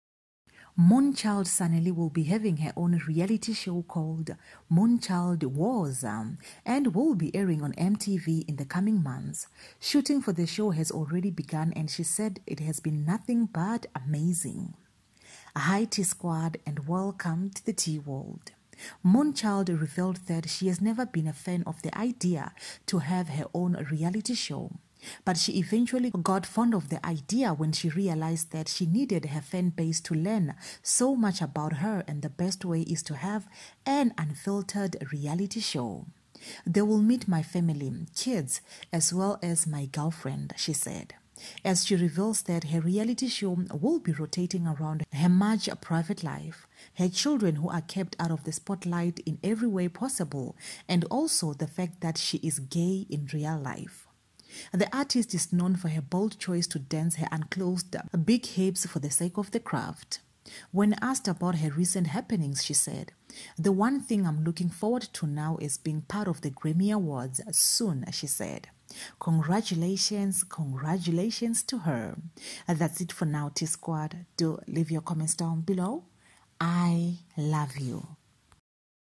WOW Congratulations To Moonchild Sanelly
Moonchild Sanelly Says We Are Yet To Meet Her Family, Her Children And Her Girlfriend.

Moon child Sanelly will be having her own reality show called Moonchild Woza' and will be airing on MTV in the coming months. Shooting for the show has already began and she said it has been nothing but "amazing."